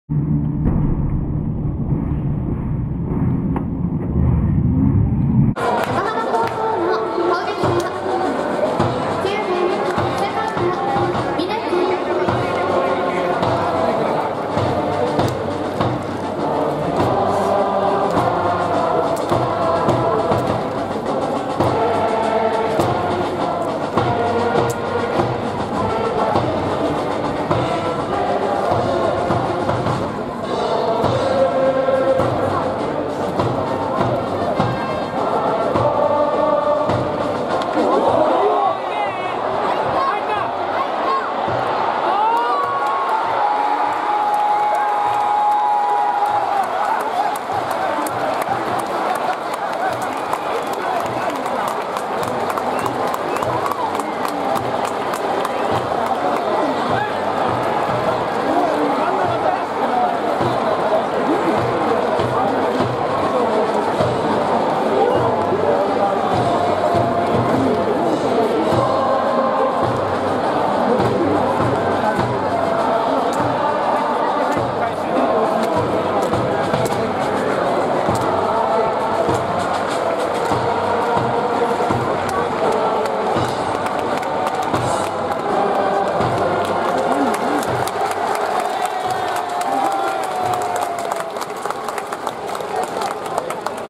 【神奈川大会決勝】横浜・峯大翔（9番打者）のホームラン【2023.7.26】
2023年7月26日 横浜スタジアム
高校野球神奈川大会（甲子園予選）決勝
横浜高校vs慶應義塾高校